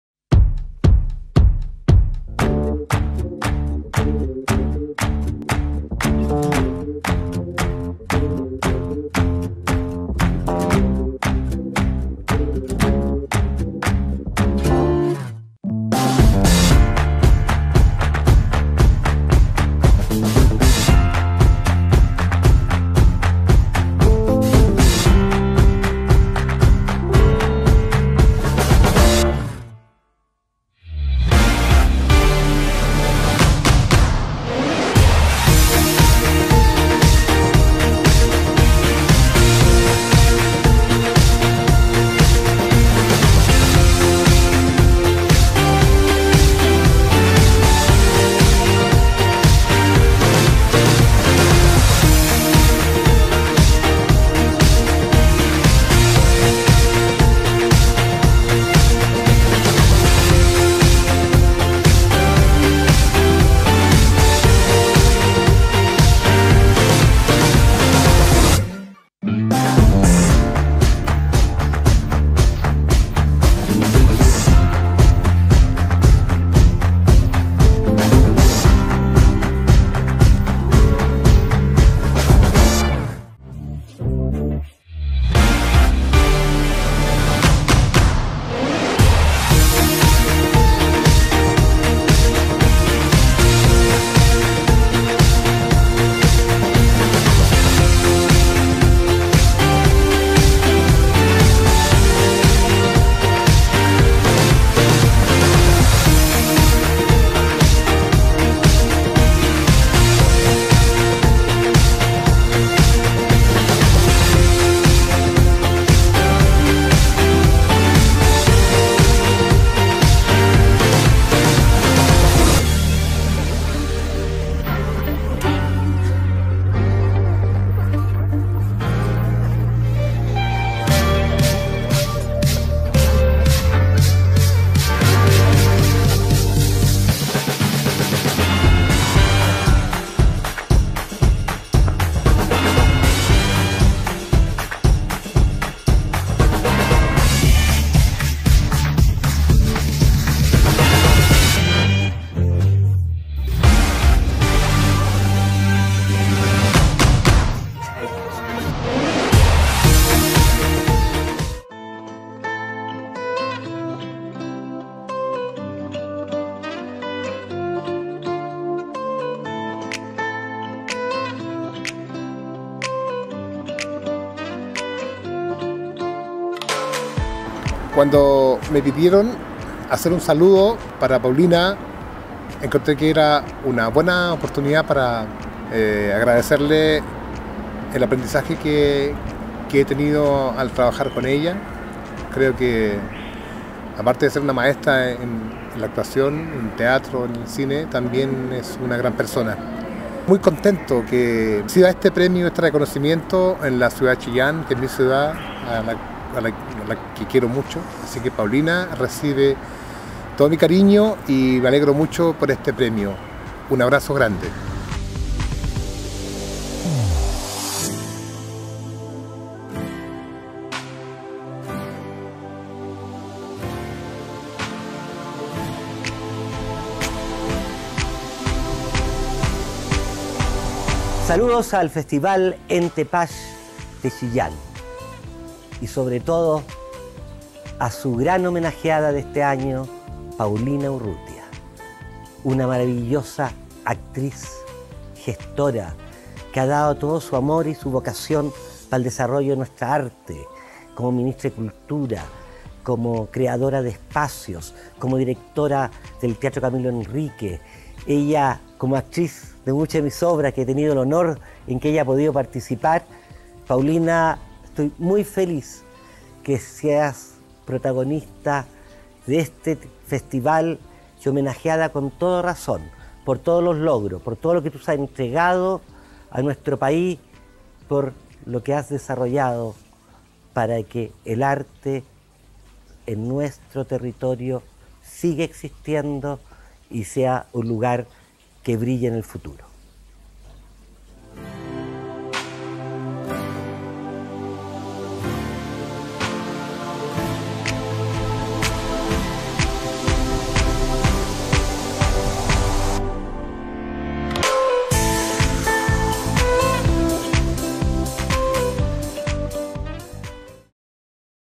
Video Homenajeada Entepach 25: Paulina Urrutia
Amigos cercanos a nuestra destacada Actriz Nacional Paulina Urrutia la saludan por el homenaje de Entepach a su trayectoria artística con el premio Enrique Gajardo

Proyecto financiado por el Ministerio de las culturas, las Artes y el Patrimonio de Chile, convocatoria 2020, línea de Difusión modalidad regional de financiamiento.